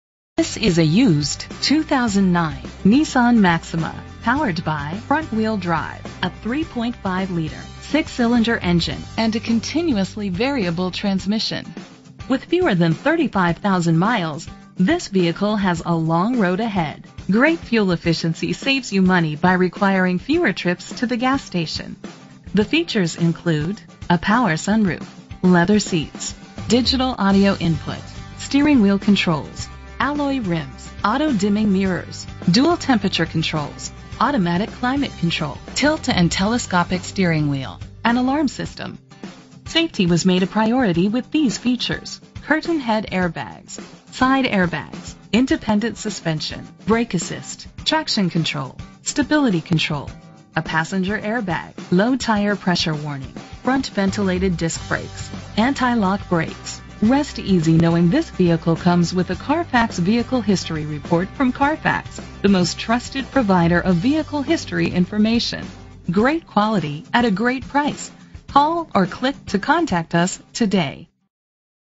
2009 Nissan Maxima - Easton PA
Phone:
Year: 2009
Make: Nissan
Model: Maxima
Trim:
Engine: 3.5 liter 6 cylinder 24 valve MPFI DOHC
Transmission: Automatic CVT
Color: Gray
Mileage: 33601
Address:
3810 Hecktown Rd.Route 33
Route 33